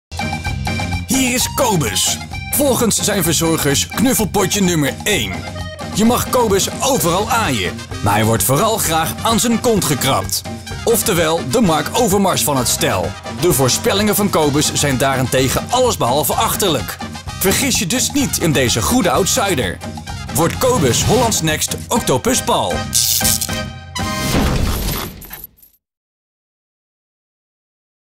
Holland's Next Octopus Paul: Kobus de Ezel
Ik denk dat Kobus de ezel Holland's Next Octopus Paul wordt!
Bekijk de voorspellingen van de twaalf kandidaten en kies jouw favoriet op Hollandsnextoctopuspaul.nl